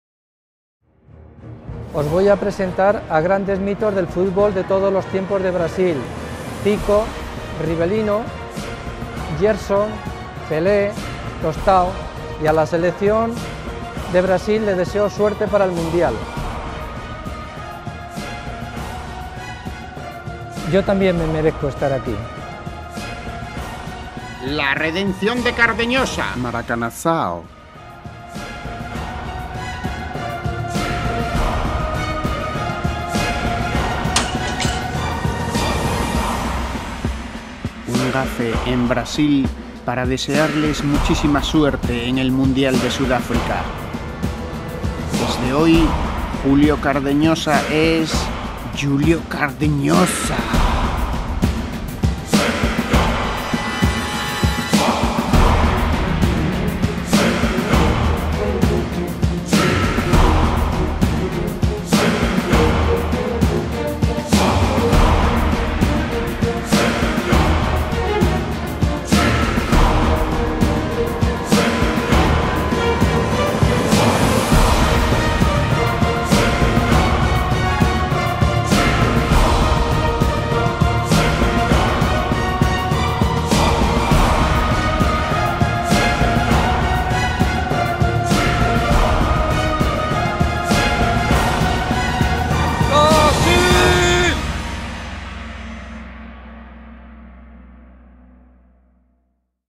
La Redención de Cardeñosa. Cap 8/9. MARACANAÇAO
Julio Cardeñosa visita el templo mundial del Futbol y su museo. El estadio de Maracaná. Ahí nuestro gafe particular intentará traspasarle toda su mala suerte a la canarinha para el Mundial de Futbol de Sudáfrica. Ahora Julio Cardeñosa es. Julio Cardenhosa. Un video de